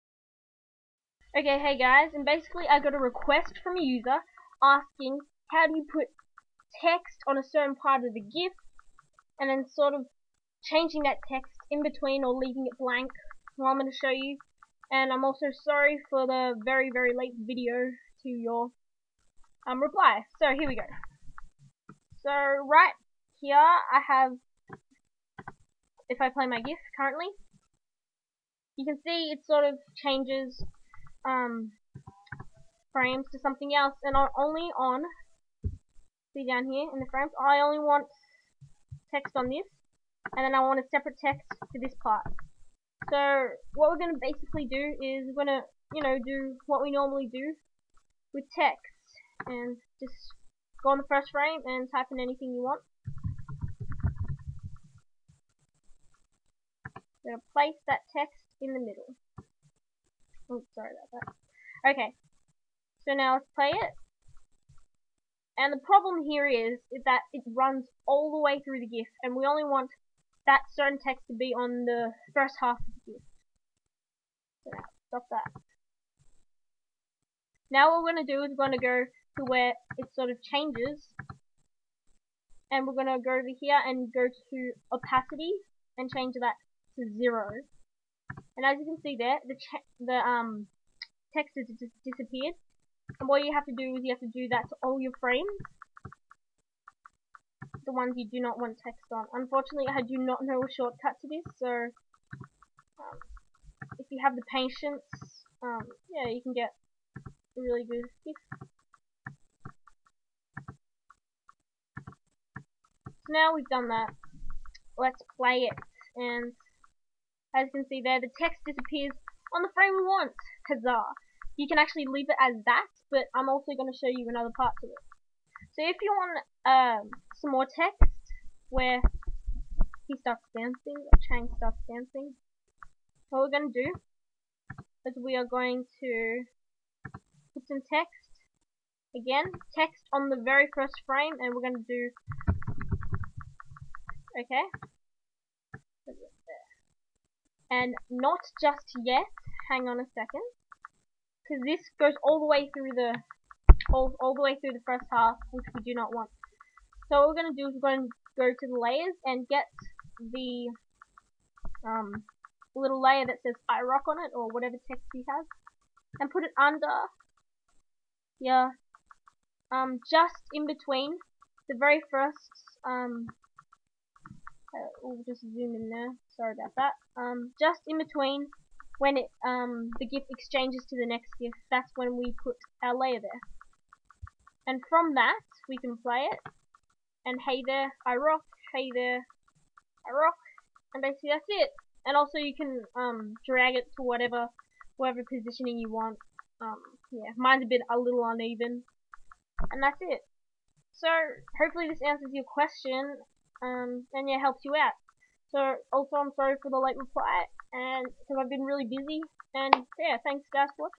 How to place text on different parts of a gif on photoshop
(REQUEST VIDEO) - Send in your requests.
How to place text on gifs at different frames